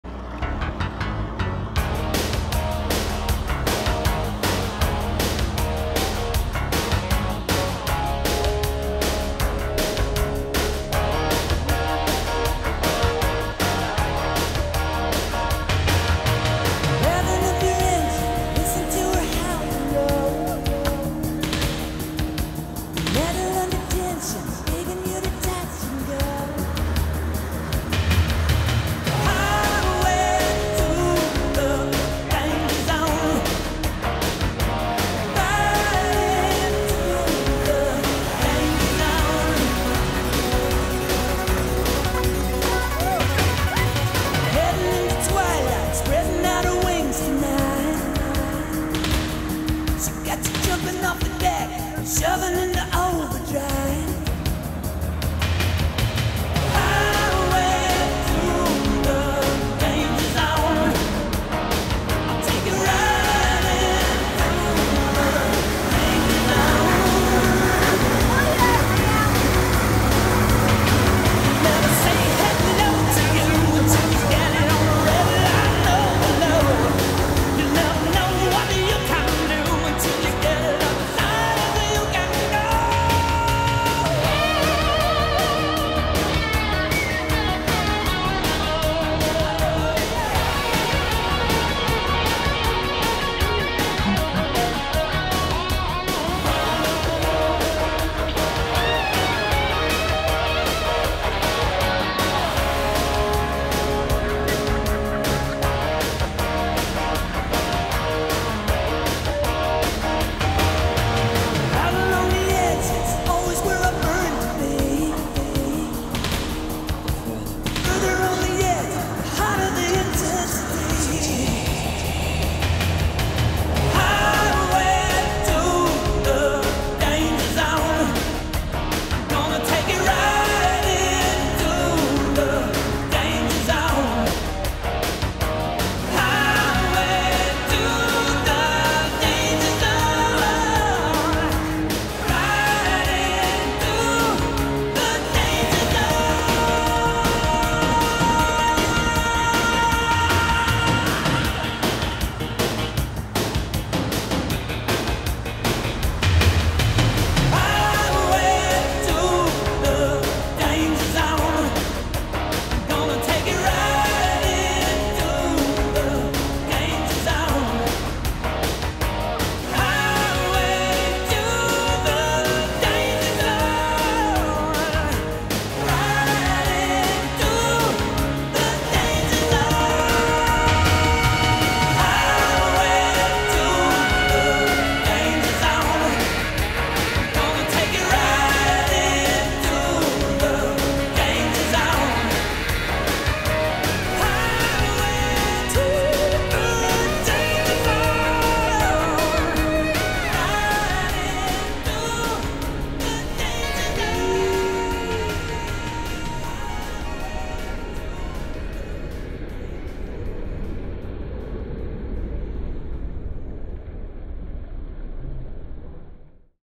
Sam's Danger Zone
Sam tries fly boarding for the first time.